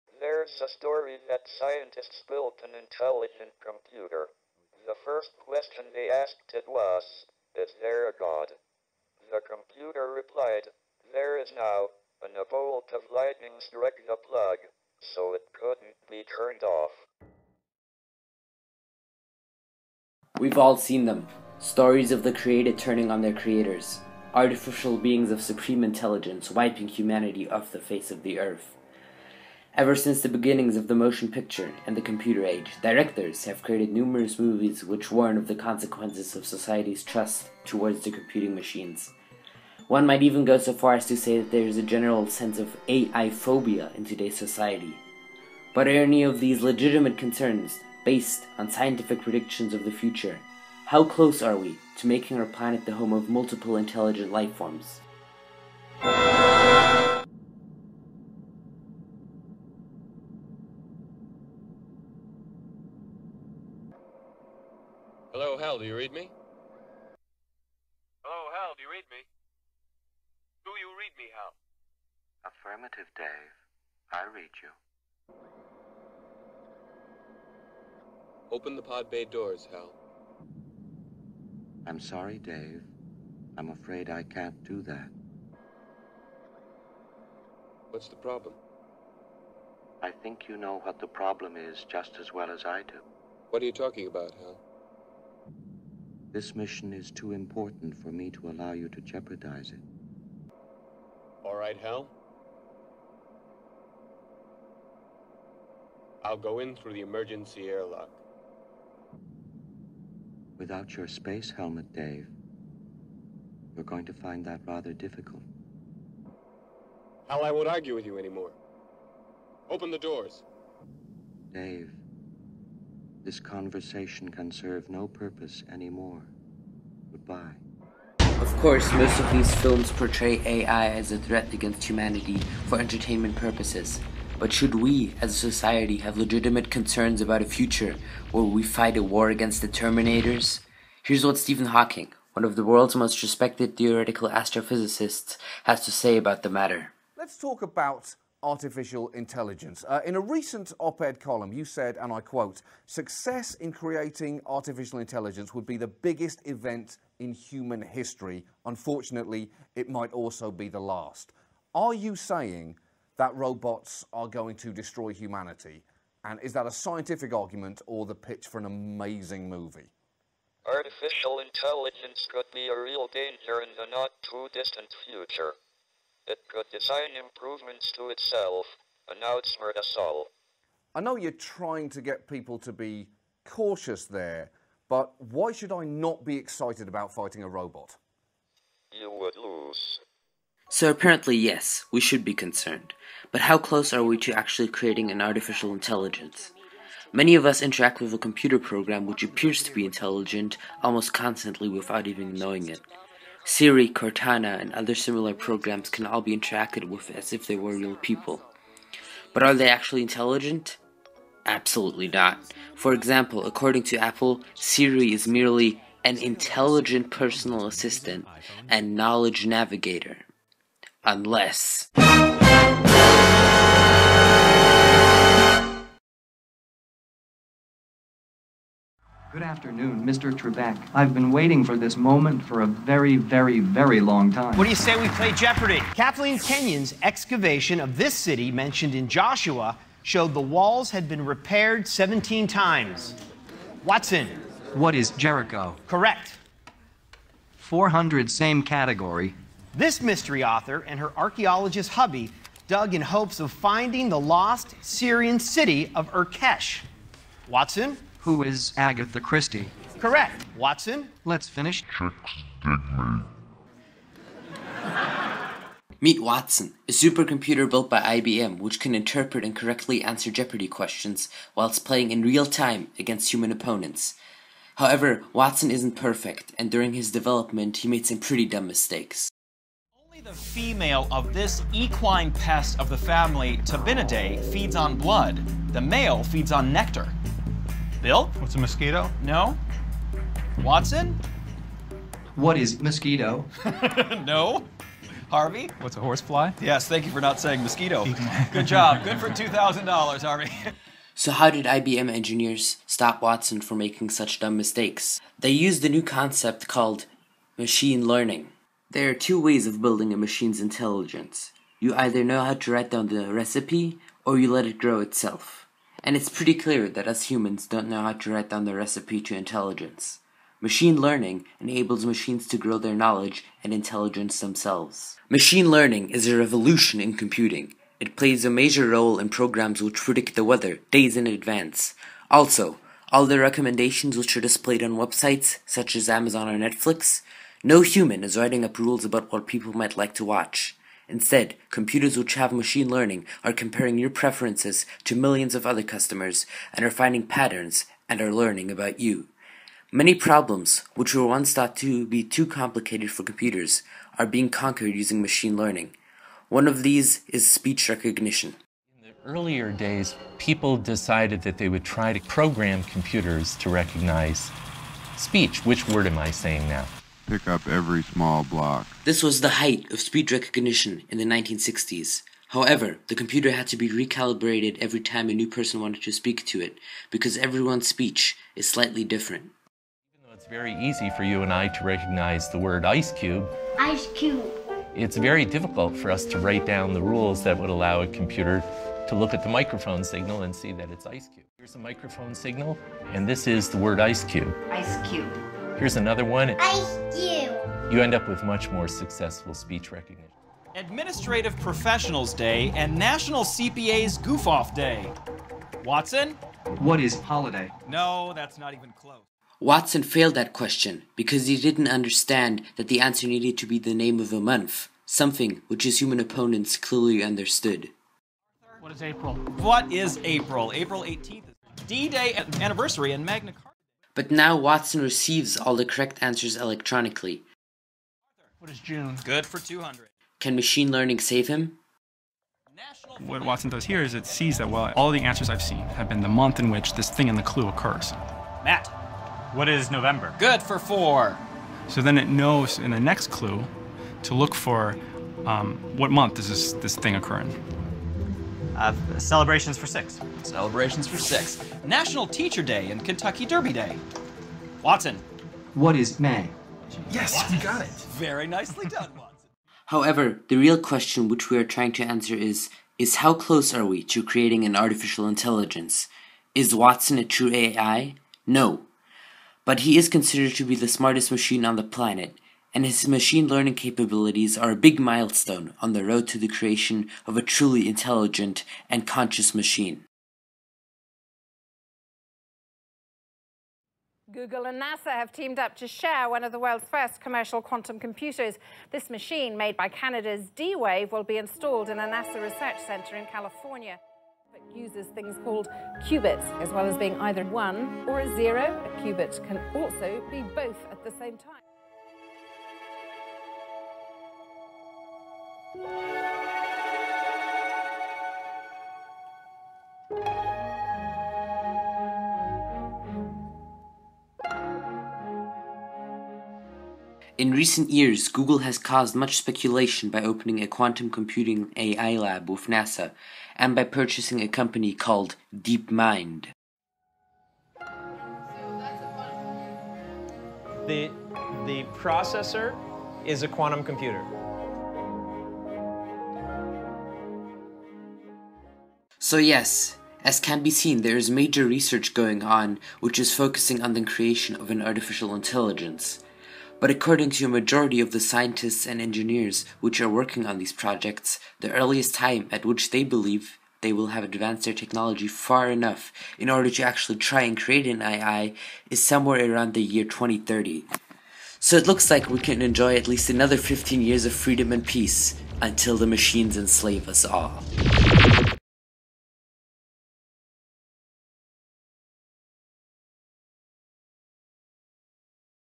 Artificial Intelligence- How close are we?
Should we as a society be concerned about artificial intelligence being a threat in the not-to-distant future? How close are we to actually creating an intelligent machine with a consciousness? Watch to find out! ;P